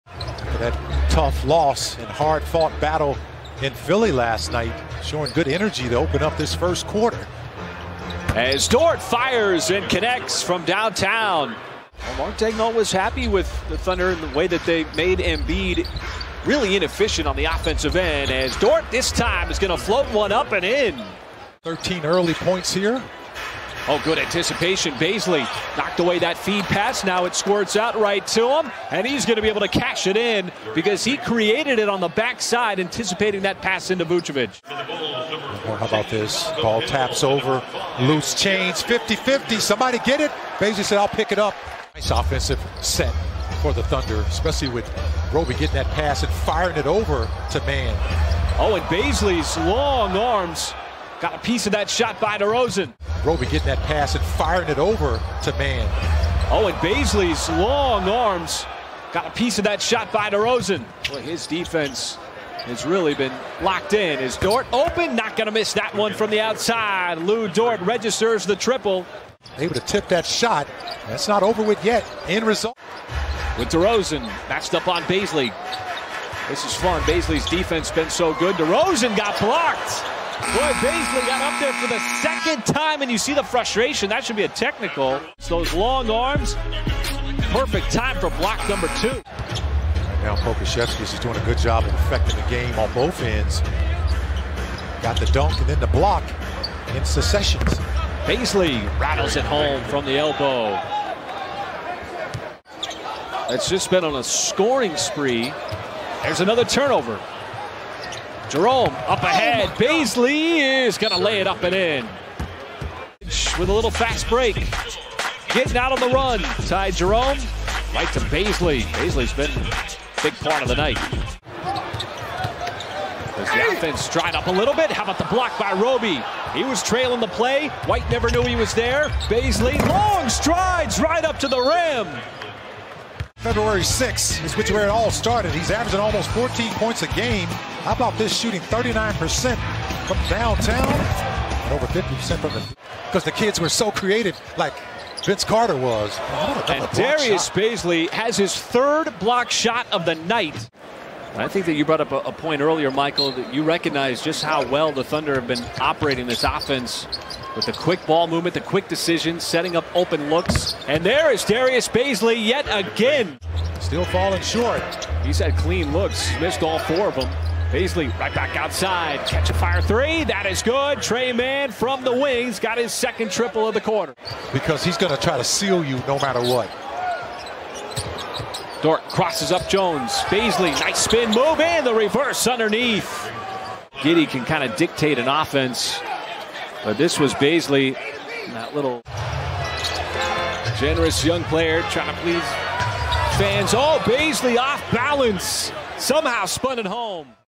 Highlights | Darius Bazley ALL-AROUND GAME at Bulls 02/12/2022
The top Baze highlights from all over the floor against the Bulls on 02/12/2022
Daruis Bazley | 17pts, 8reb, 5ast, 2stl, 3blk